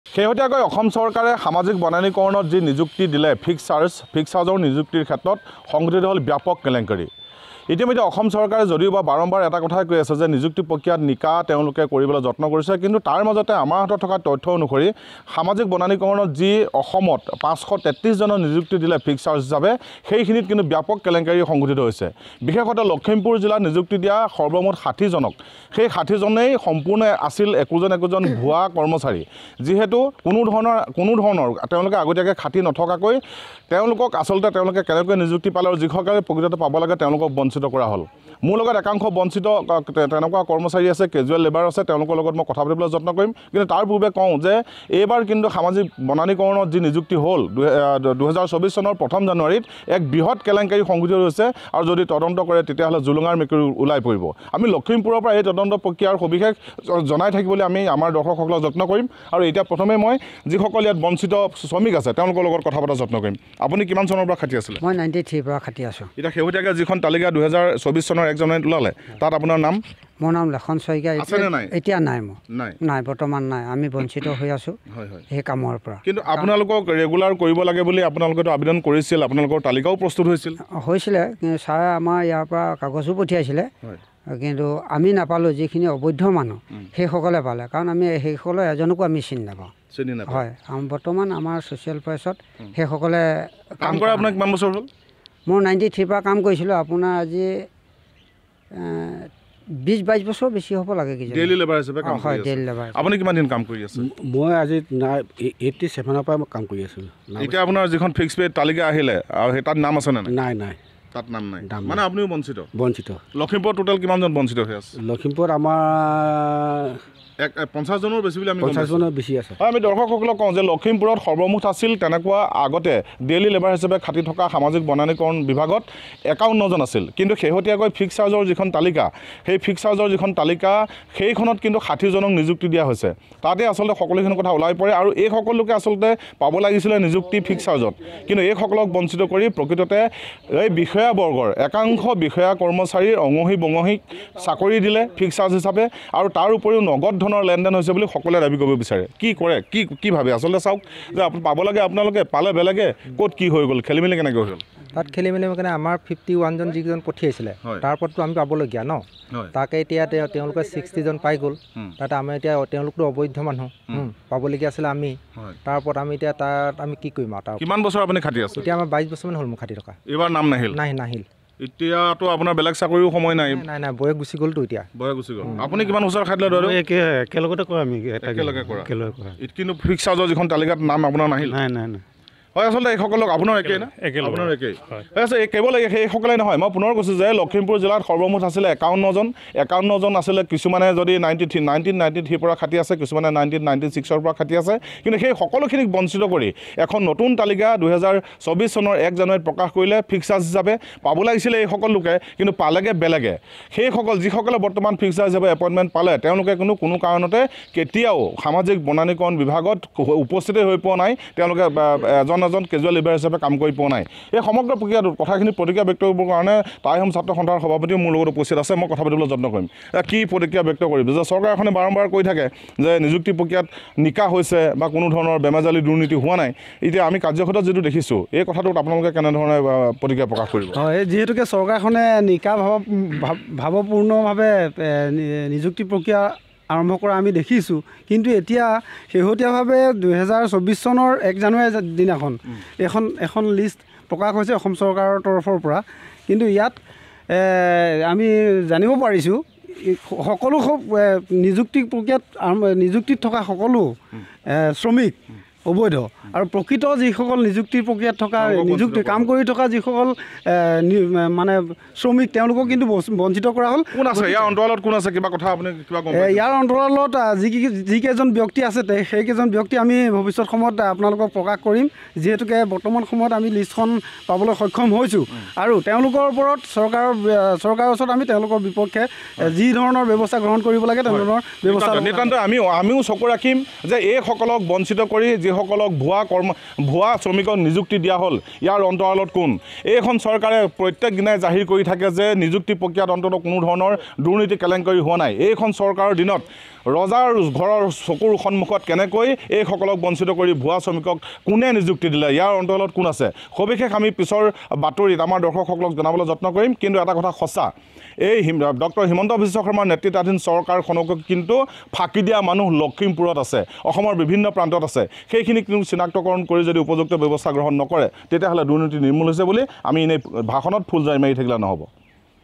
Appointment Scam In Forest Department Lakhimpur | সামাজিক বনানিকৰণৰ নিযুক্তিত ভয়ংকৰ কেলেংকাৰী N18V
Appointment Scam In Forest Department Lakhimpur |  সামাজিক বনানিকৰণৰ নিযুক্তিত ভয়ংকৰ কেলেংকাৰী।
২০২৪চনৰ ১জানুৱাৰৰীত ৰাজ্য চৰকাৰে সামাজিক বনানীকৰণত নিযুক্তি দিয়া ৫৩৩ জন ফিক্সড পে কৰ্মচাৰীৰ অধিকাংশই ভুৱা কৰ্মচাৰী বুলি অভিযোগ আনিছে লখিমপুৰৰ একাংশ লোকে।